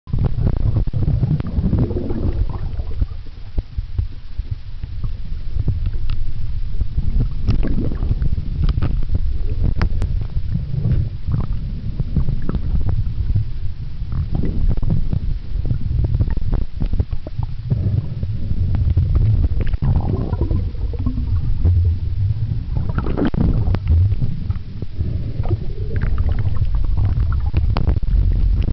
Vi e JP Mergulhando em Porto
Vi e JP Mergulhando em Porto de Galinhas
em Janeiro de 2006